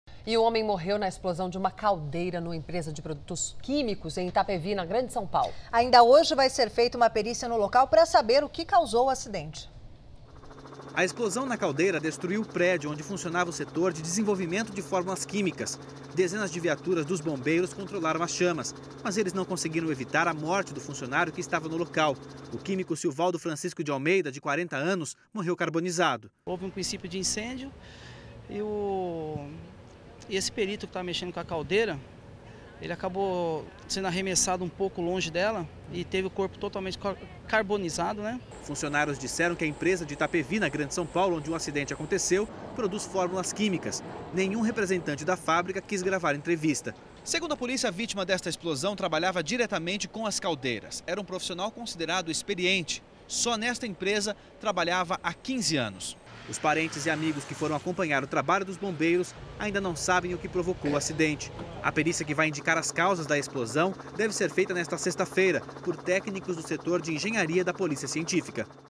Explosão Caldeira Fabrica Produto Químico
Caldeira de empresa de produtos químicos explode e mata funcionário em Itapevi (SP)